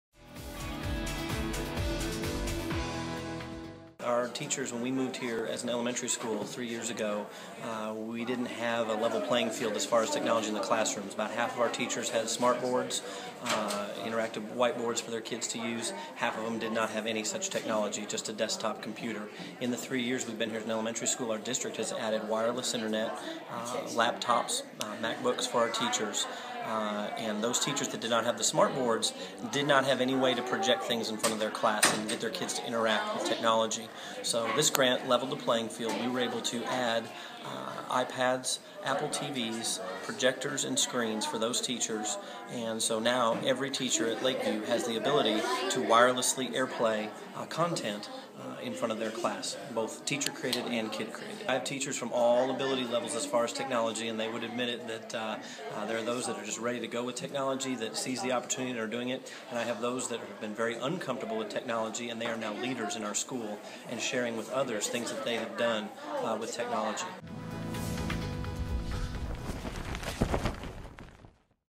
OETT grants making a difference in schools (2014-01-24)
OETT grants help Lakeview Elementary in supplying 30 iPads, smart boards and projection systems for many of its classrooms. The school currently serves about 600 students.